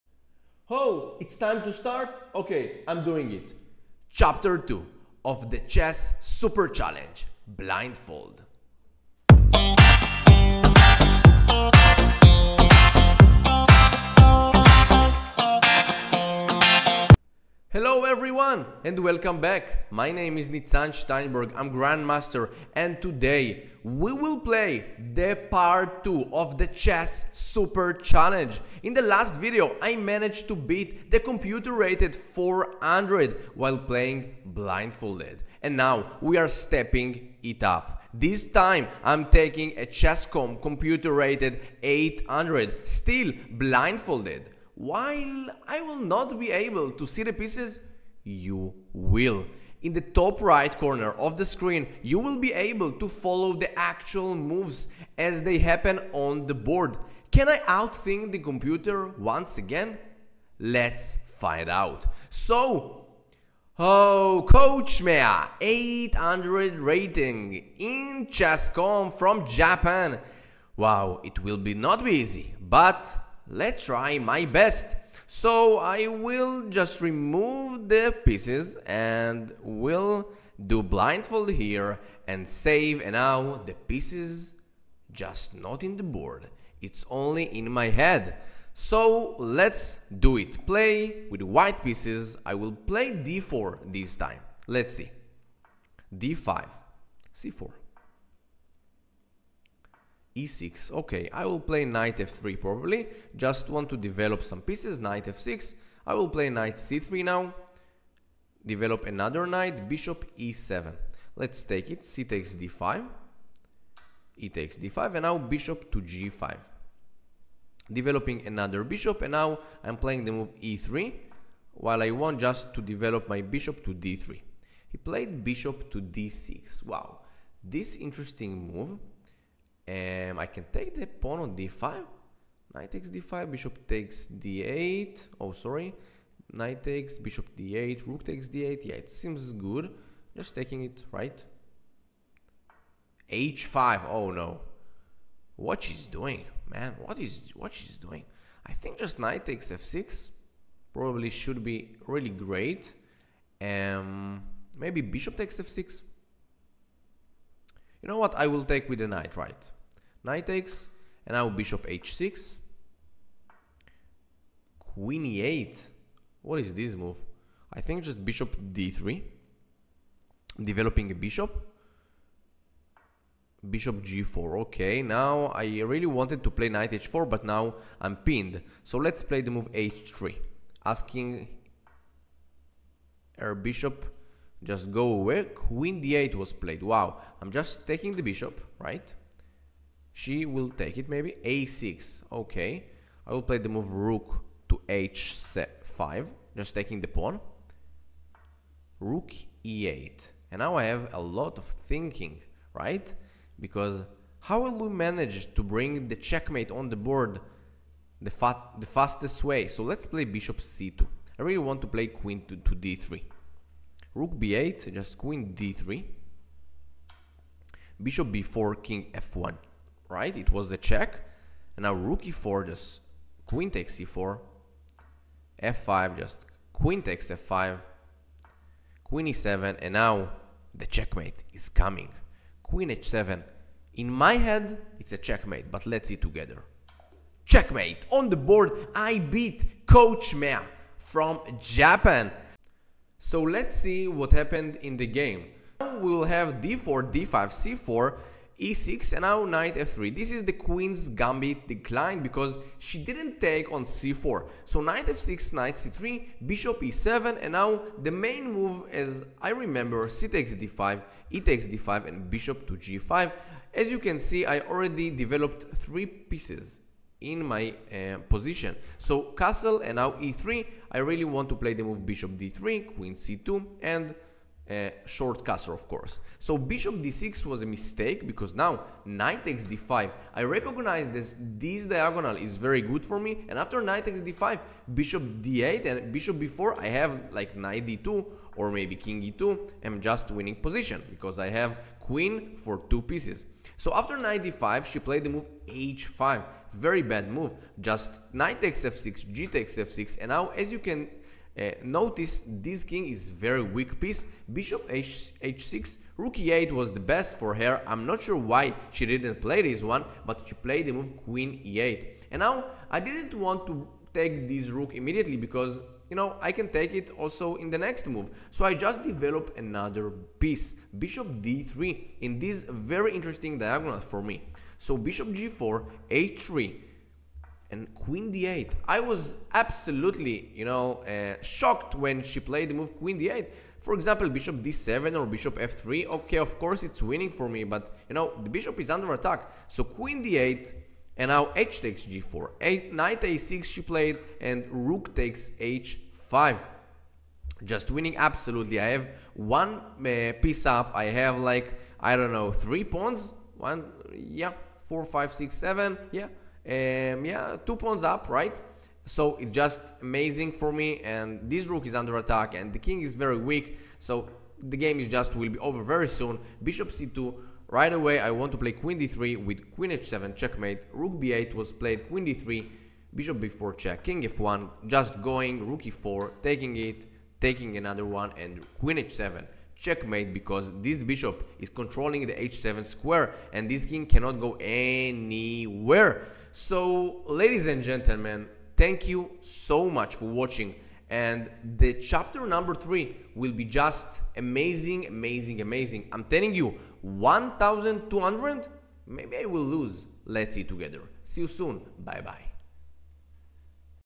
The Chess Super Challenge - BLINDFOLD !!! #2
Welcome to the Second episode of the Chess Super Challenge series! In this video, I take on a chess.com computer rated 80, but with a crazy twist – I’m playing blindfolded, with all the pieces invisible! Watch as I try to navigate the board using only my memory and visualization. Can I defeat the computer without seeing a single piece? Think you can beat the blindfold challenge?

🔹Enjoy my videos?